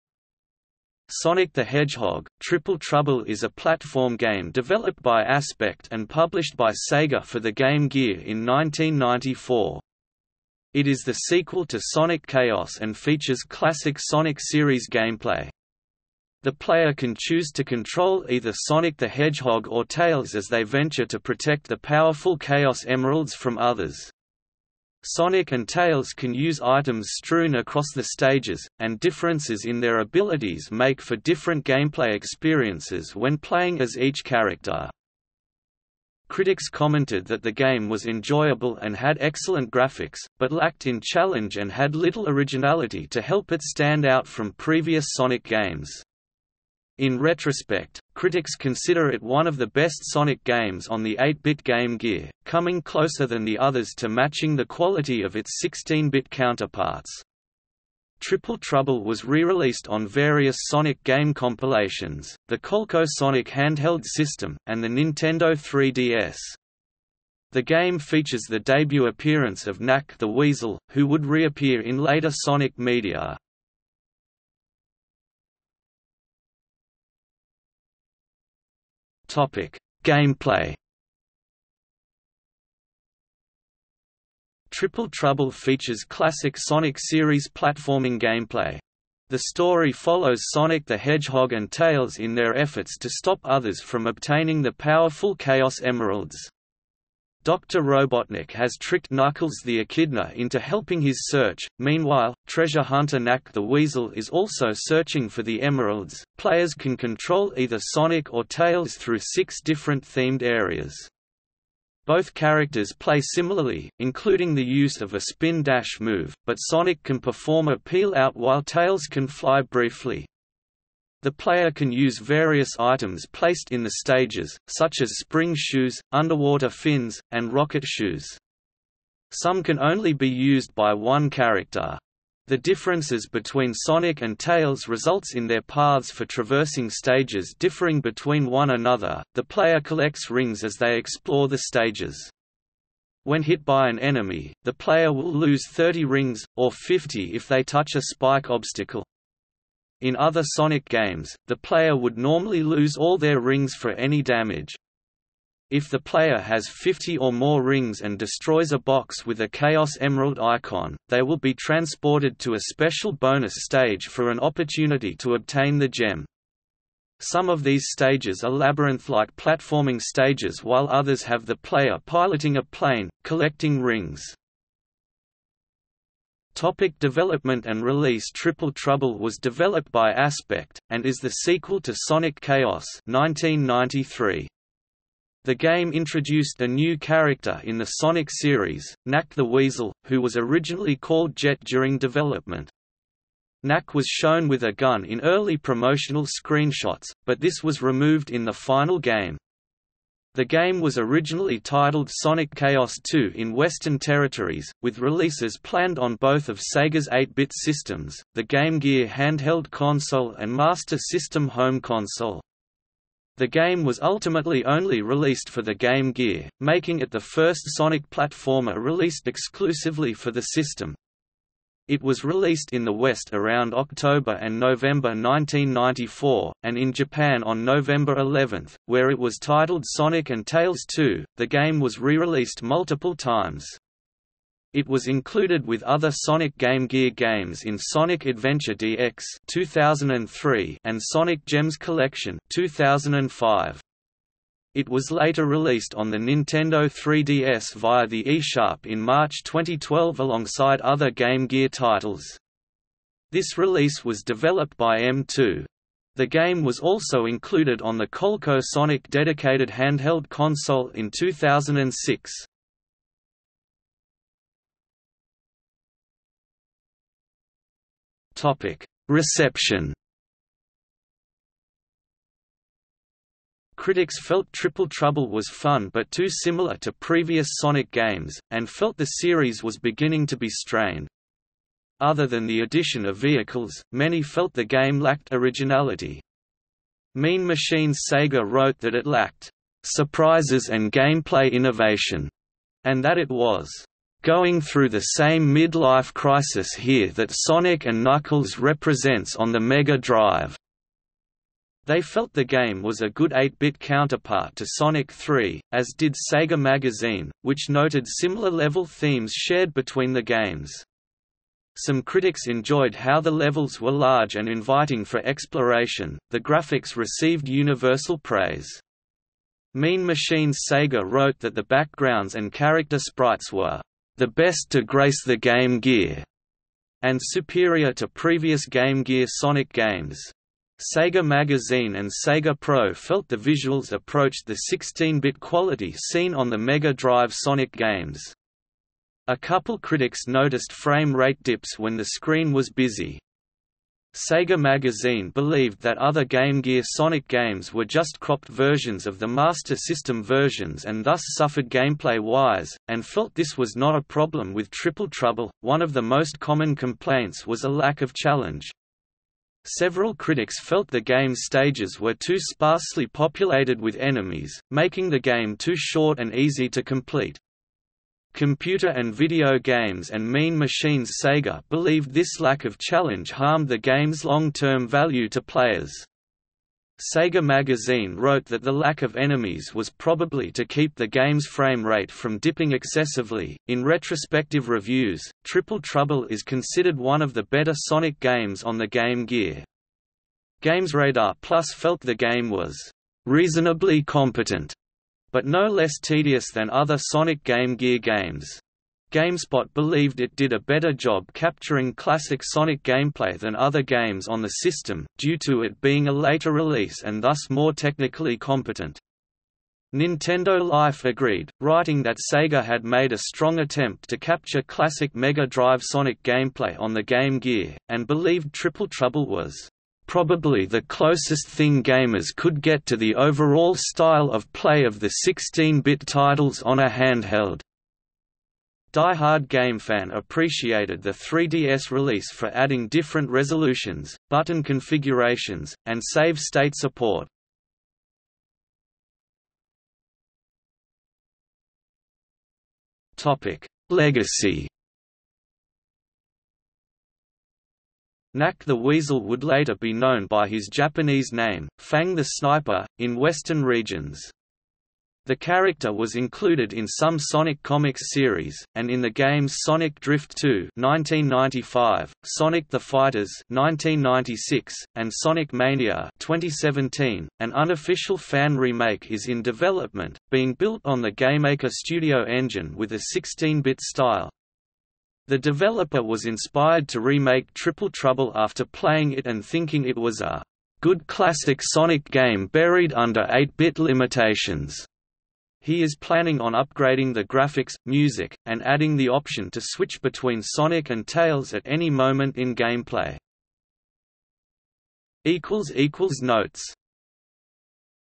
Sonic the Hedgehog: Triple Trouble | Wikipedia audio article
00:01:15 1 Gameplay
00:02:56 2 Development and release
00:04:24 3 Reception
00:07:19 4 Legacy

- Socrates

SUMMARY
Sonic the Hedgehog: Triple Trouble is a platform game developed by Aspect and published by Sega for the Game Gear in 1994. It is the sequel to Sonic Chaos and features classic Sonic series gameplay. The player can choose to control either Sonic the Hedgehog or Tails as they venture to protect the powerful Chaos Emeralds from others. Sonic and Tails can use items strewn across the stages, and differences in their abilities make for different gameplay experiences when playing as each character.
Critics commented that the game was enjoyable and had excellent graphics, but lacked in challenge and had little originality to help it stand out from previous Sonic games. In retrospect, critics consider it one of the best Sonic games on the 8-bit Game Gear, coming closer than the others to matching the quality of its 16-bit counterparts. Triple Trouble was rereleased on various Sonic game compilations, the Coleco Sonic handheld system, and the Nintendo 3DS. The game features the debut appearance of Nack the Weasel, who would reappear in later Sonic media.